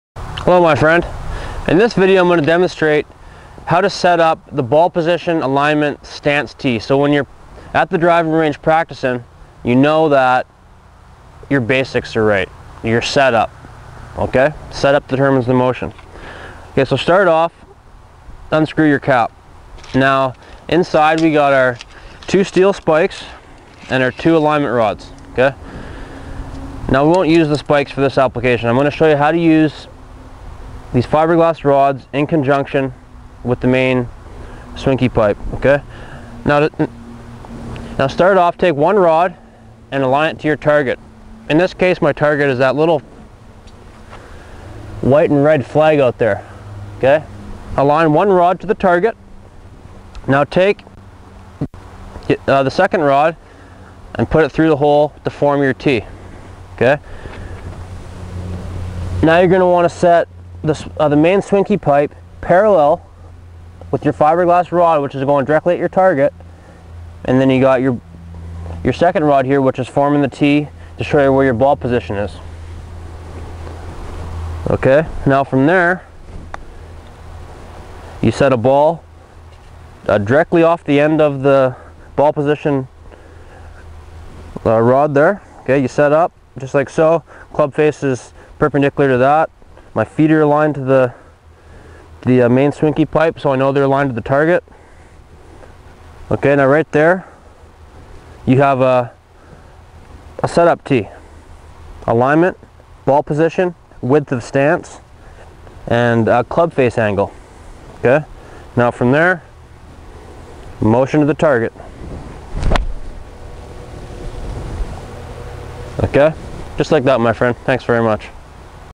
The Swinkey Academy - Setup Instructions
Brian Benedictson demonstrates how the Swinkey is used to monitor and improve your setup. The setup consists of ball position, alignment, stance and posture.